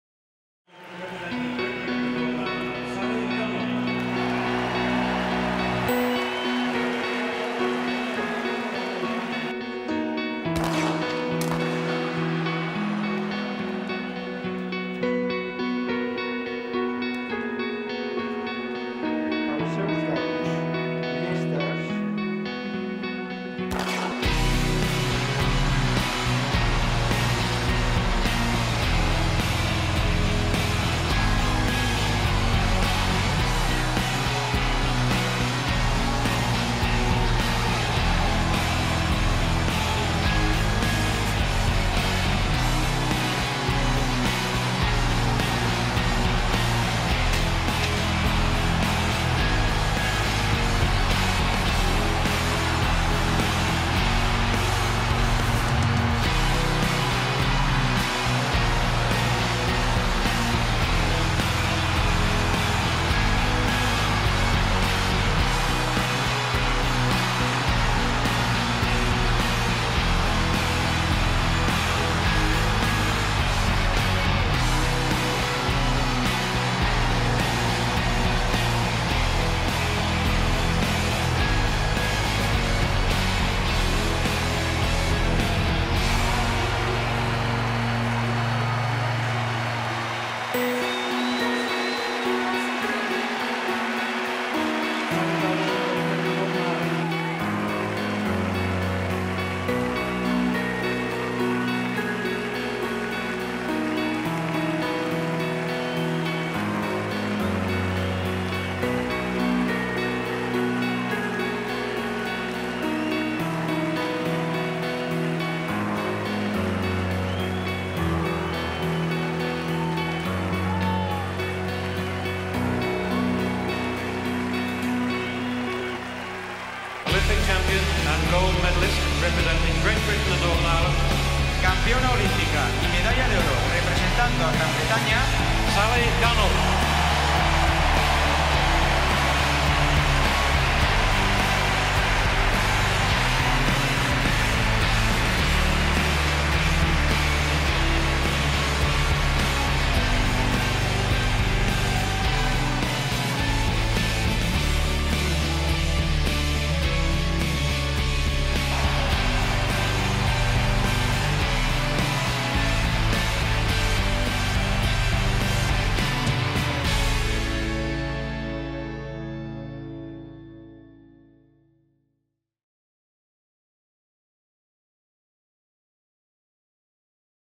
Sally Gunnell wins 400m Hurdles Gold - Barcelona 1992 Olympics
Sally Jane Janet Gunnell attended West Hatch High School in Chigwell, Essex, east of London. Long Jump and the Pentathlon were her sports before she graduated to sprinting and the hurdles that would change her life.